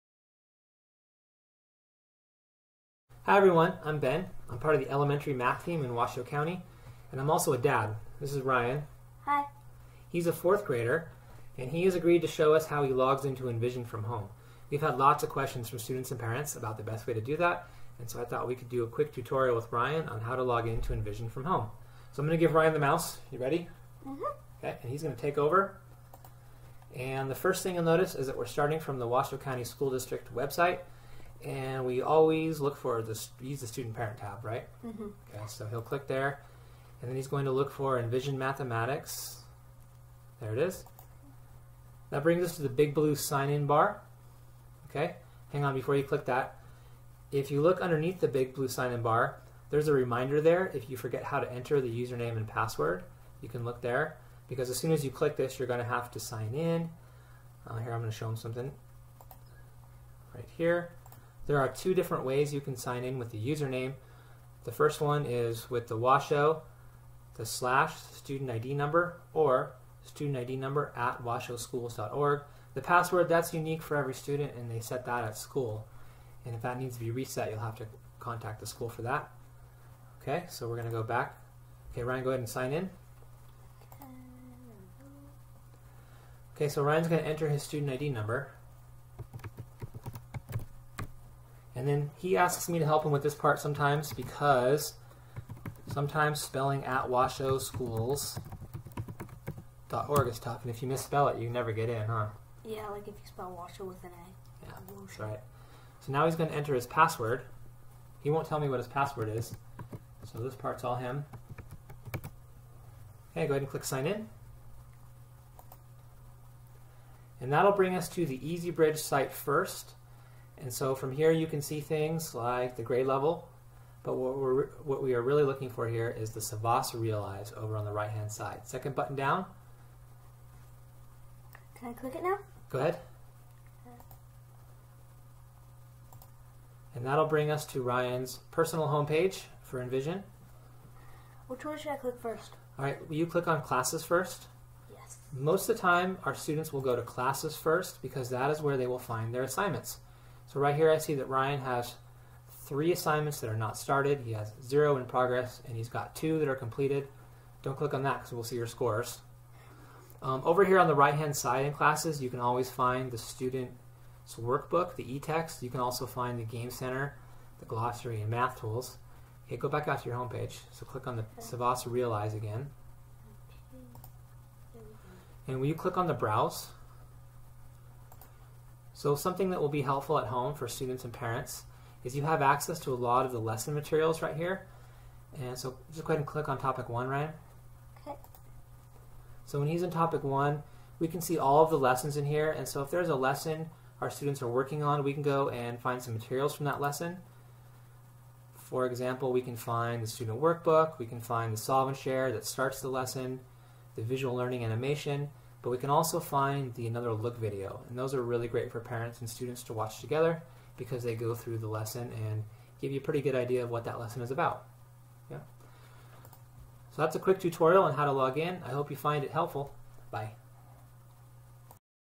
How to log in to envision math
Tutorial video on how students log in to their personal envision accounts from a device when not at school.